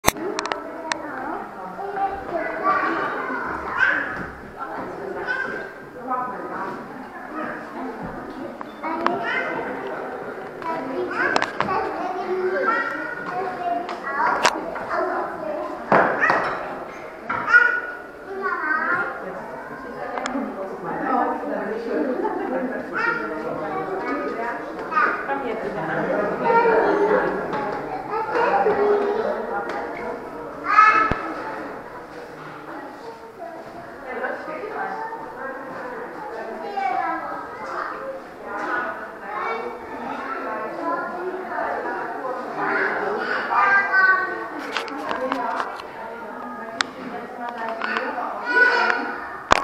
Fossilen - Sammlung , Museum Braunschweig 2019 ; 4 K
Im  Naturhistorischen  Museum  (  beim  Campus  )   werden  reichlich
Fossilien  gezeigt  .